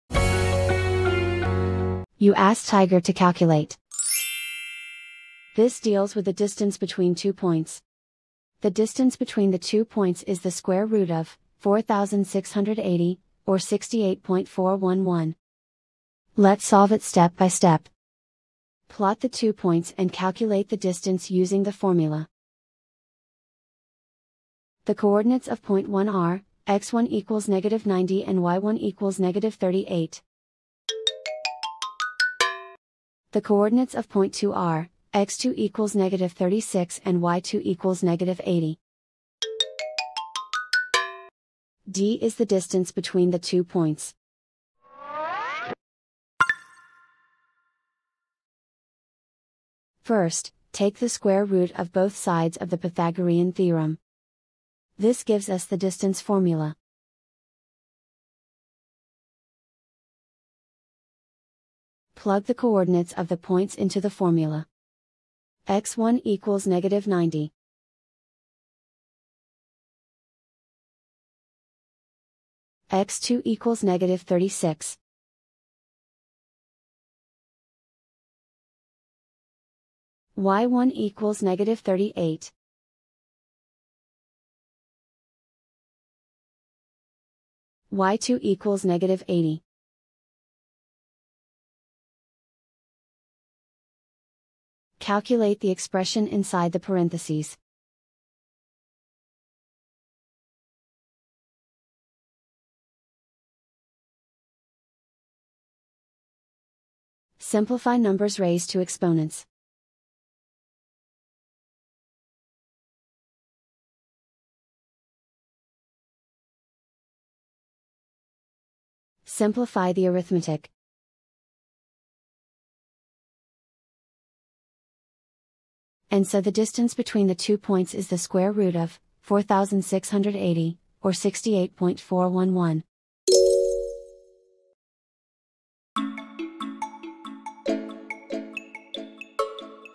Find the distance between two points p1 (-90,-38) and p2 (-36,-80): Step-by-Step Video Solution
See how to easily find the distance between two points p1 (-90,-38) and p2 (-36,-80) with our clear, step-by-step video guide on Tiger Algebra. Also, check out for more tips and tricks